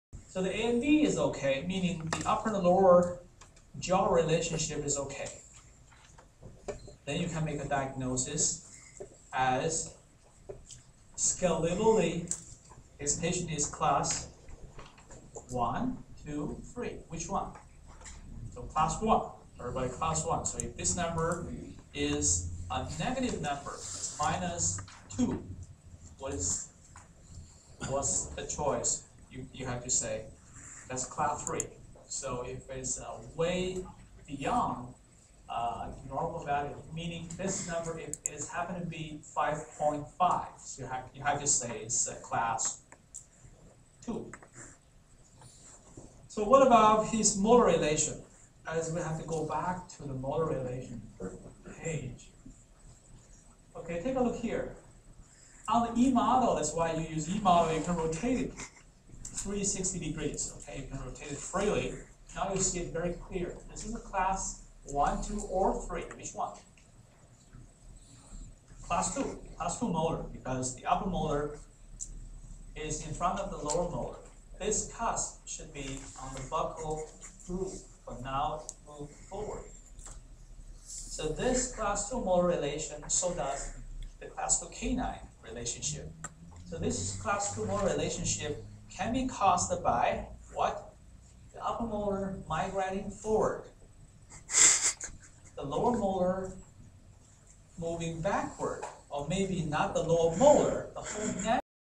student agrees w. everything prof says in an enthralling lecture.wmv
"duuude, i'm not sleeping. i'm just resting my eyes."